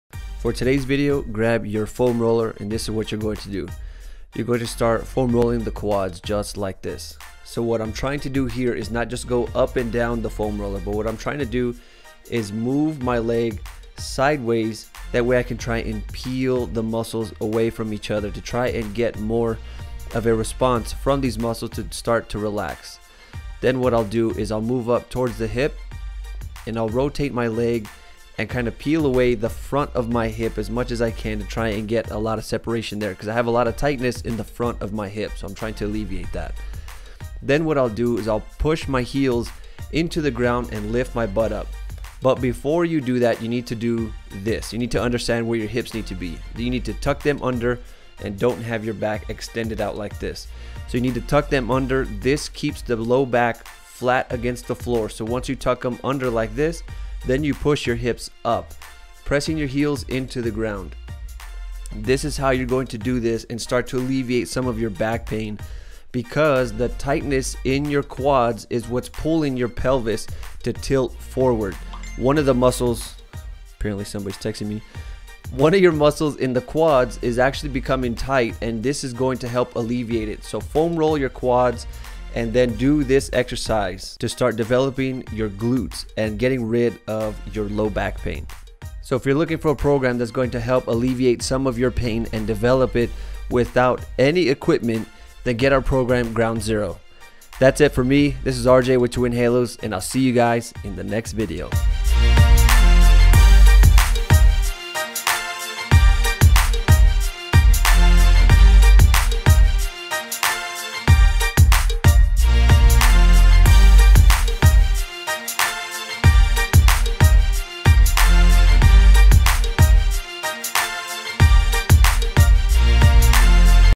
Motocross Riders Can Reduce Low Back Pain At Home NOW!
Foam rolling to alleviate back pain

Here is one technique to use when foam rolling. To follow the soft tissue work there is also an exercise you need to do to balance out the work you just did.

Follow the steps here in the video so that you know how to place your pelvis when performing the exercise.

Are you wanting to know how to weight train for motocross? Iron foundation is our New downloadable program that can help you get started today.

My foam roller
My lacrosse ball